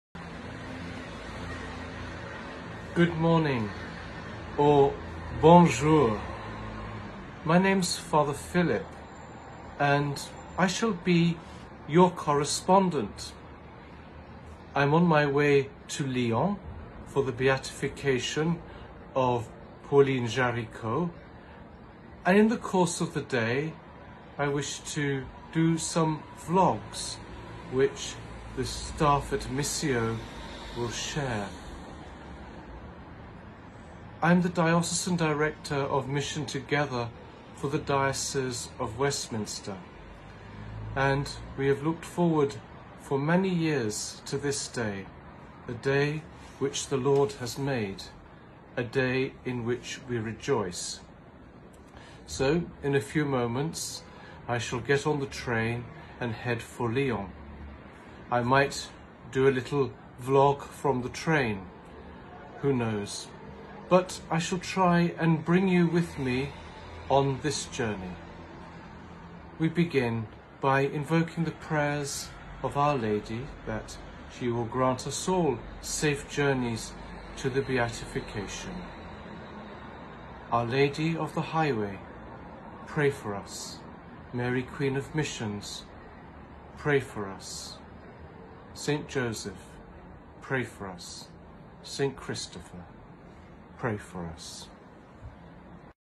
Fr Philip Baptiste: Vlogs from the Beatification of Pauline Jaricot (1)
Fr Philip Baptiste, the Westminster Diocesan Director for Missio's children's branch - Mission Together - was in France to celebrate the beatification of one of our founders, Pauline Jaricot. Fr Philip was our 'eyes on the ground' and shared vlogs with us throughout the day. Watch now as he makes his way from Paris to Lyon, and join us in the celebrations.